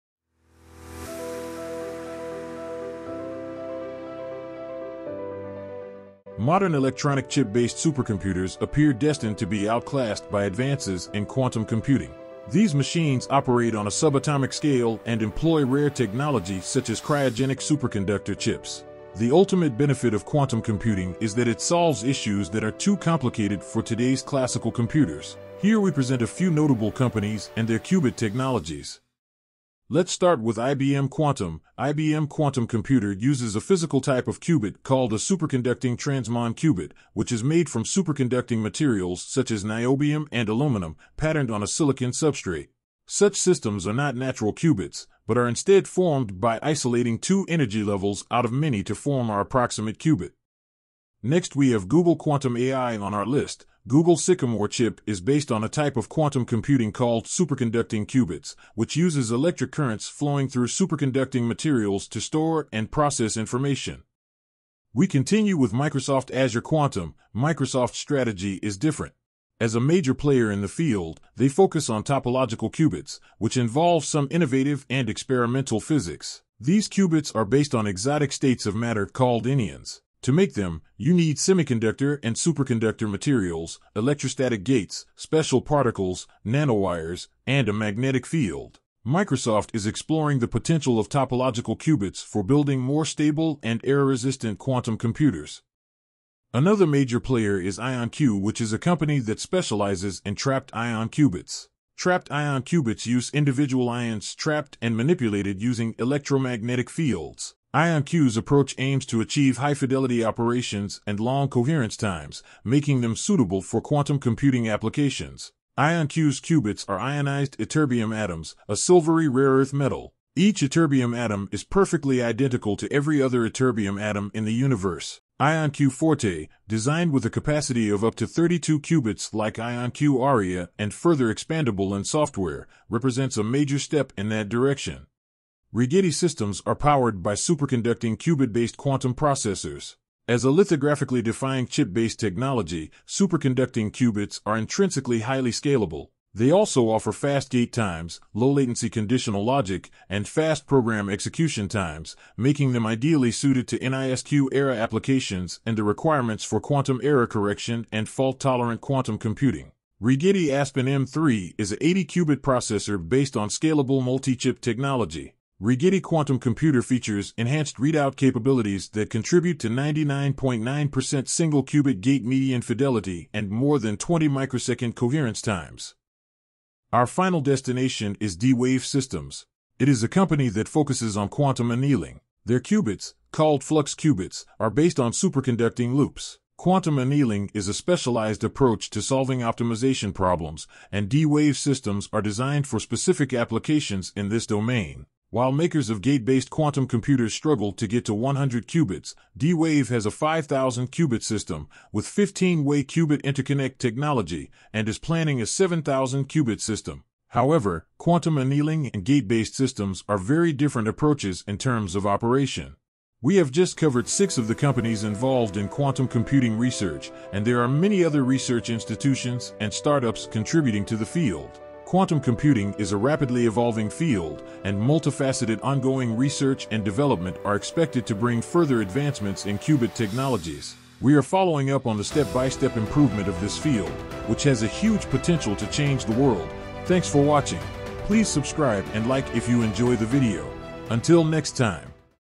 Quantum Computers: Industry Pioneers and Qubit Technologies
Modern electronic chip-based supercomputers appear destined to be outclassed by advances in quantum computing. These machines operate on a subatomic scale and employ rare technology such as cryogenic superconductor chips. The ultimate benefit of quantum computing is that it solves issues that are too complicated for today's classical computers. Here we present a few notable companies and their qubit technologies: Let’s start with IBM Quantum: IBM Quantum computer uses a physical type of qubit called a superconducting transmon qubit, which is made from superconducting materials such as niobium and aluminum, patterned on a silicon substrate. Such systems are not natural qubits, but are instead formed by isolating two energy levels out of many to form our approximate qubit. Next we have Google Quantum AI on our list: Google Sycamore chip is based on a type of quantum computing called superconducting qubits, which uses electric currents flowing through superconducting materials to store and process information. We continue with Microsoft Azure Quantum: Microsoft's strategy is different. As a major player in the field, they focus on topological qubits, which involves some innovative and experimental physics. These qubits are based on exotic states of matter called anyons. To make them, you need semiconductor and superconductor materials, electrostatic gates, special particles, nanowires, and a magnetic field. Microsoft is exploring the potential of topological qubits for building more stable and error-resistant quantum computers. Another major player is IonQ which is a company that specializes in trapped ion qubits. Trapped ion qubits use individual ions trapped and manipulated using electromagnetic fields. IonQ's approach aims to achieve high-fidelity operations and long coherence times, making them suitable for quantum computing applications. IonQ's qubits are ionized ytterbium atoms, a silvery rare-earth metal. Each ytterbium atom is perfectly identical to every other ytterbium atom in the universe. IonQ Forte, designed with a capacity of up to 32 qubits like IonQ Aria and further expandable in software, represents a major step in that direction. Rigetti systems are powered by superconducting qubit-based quantum processors. As a lithographically defined chip-based technology, superconducting qubits are intrinsically highly scalable. They also offer fast gate times, low-latency conditional logic, and fast program execution times, making them ideally suited to NISQ era applications and the requirements for quantum error correction and fault-tolerant quantum computing. Rigetti Aspen-M-3 is a 80-qubit processor based on scalable multi-chip technology. It features enhanced readout capabilities that contribute to 99.9 % single-qubit gate median fidelity and more than 20-microsecond coherence times. Our final destination is D-Wave Systems. It is a company that focuses on quantum annealing. Their qubits, called flux qubits, are based on superconducting loops. Quantum annealing is a specialized approach to solving optimization problems, and D-Wave's systems are designed for specific applications in this domain. While makers of gate-based quantum computers struggle to get to 100 qubits, D-Wave has a 5000-qubit system, with 15-way qubit interconnect technology, and is planning a 7000-qubit system. However, quantum annealing and gate-based systems are very different approaches in terms of operation. We have just covered six of the companies involved in quantum computing research, and there are many other research institutions and start-ups contributing to the field. Quantum computing is a rapidly evolving field, and multifaceted ongoing research and development are expected to bring further advancements in qubit technologies. We are following up on the step-by-step improvement of this field, which has a huge potential to change the world, along with artificial intelligence. Until next time.